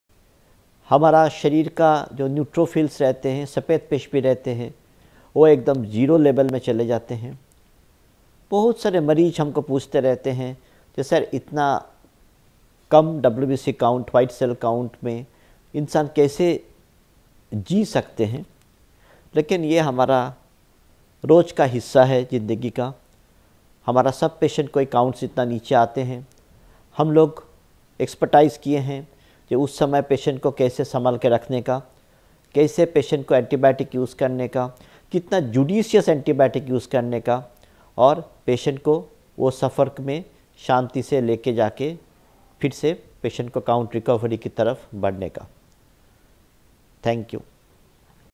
Is your White Blood Cell are very low? | शरीर में WBC कम होना | White blood cell kaise banaye#shorts
Is your White Blood Cell are very low? | शरीर में WBC कम होना | White blood cell kaise banaye?
Is your White Blood Cell are very low?

Understand the implications of low white blood cell counts in "Is your White Blood Cell are very low?" The video aims to empower viewers without over-glorifying the doctor, providing guidance on managing this medical concern.